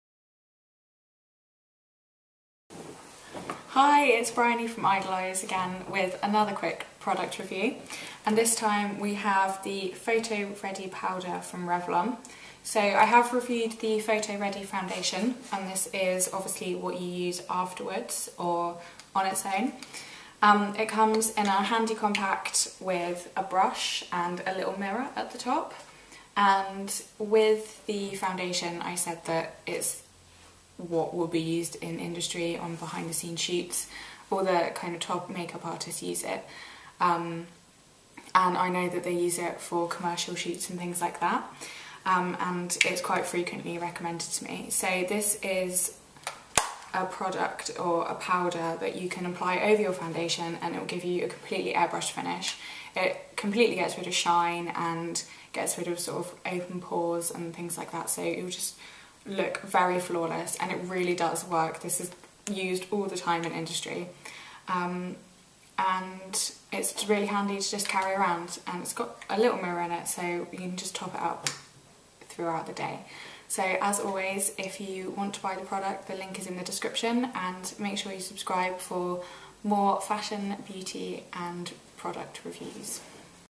Revlon PhotoReady Powder
We do a quick review on the Revlon PhotoReady Powder. Essentially we think its fabulous! Revlon is used by absolutely everyone in the movie, fashion, beauty industries to name just a few.